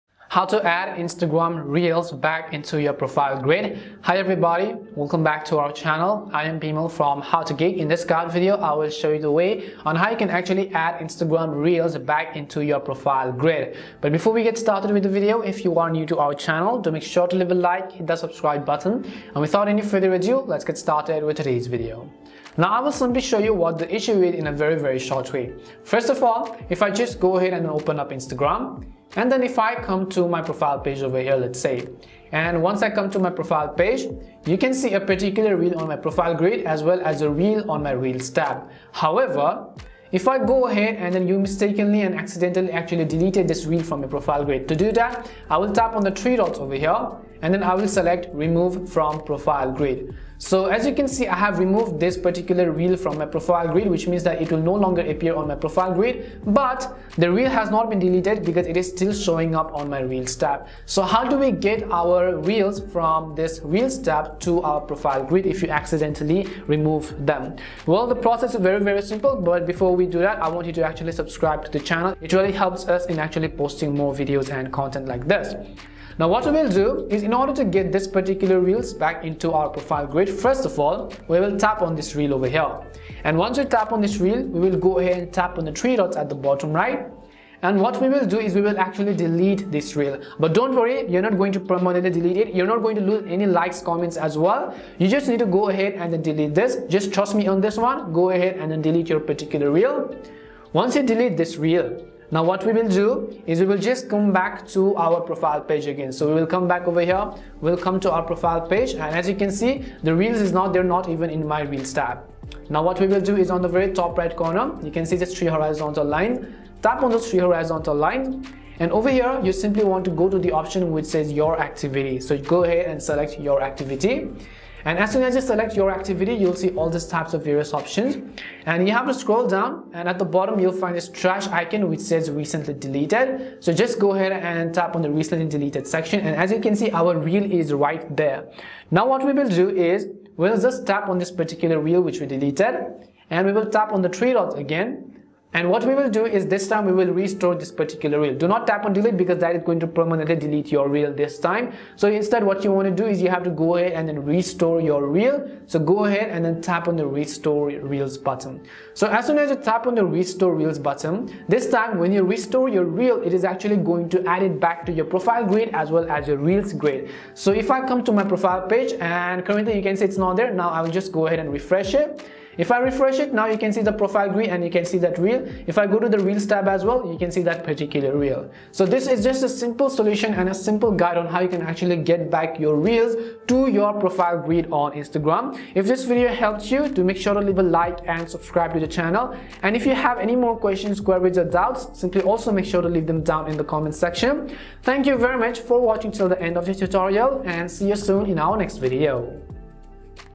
How to Add Reel Back to Profile Grid | Put Reels Back on Instagram (2022)
In this tutorial video, I will quickly guide you on how you can add a reel back to the profile grid. So make sure to watch this video till the end.

1) Update the Instagram app to the latest version
2) Open Instagram and log in to your account
3) Go to your profile page
4) Delete the reel you want to restore to your profile grid
5) Now tap on the three lines on the top right
6) Go to Your Activity
7) Go to Recently Deleted
8) Restore that particular reel you deleted earlier
9) This time when you restore it, it will be added back into the profile grid.

0:00 Introduction
0:23 What the Issue Is
1:07 How to Add Reel Back to Profile Grid
2:59 Outro: Final Verdict